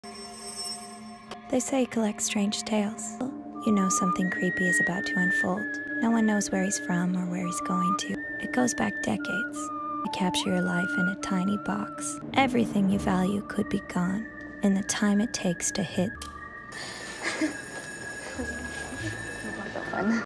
ASFR - Magic Camera Freezes/Unfreezes All Activity at School
The weird entity utilizes a supernatural camera that can stop time. This is a super cool example of its use!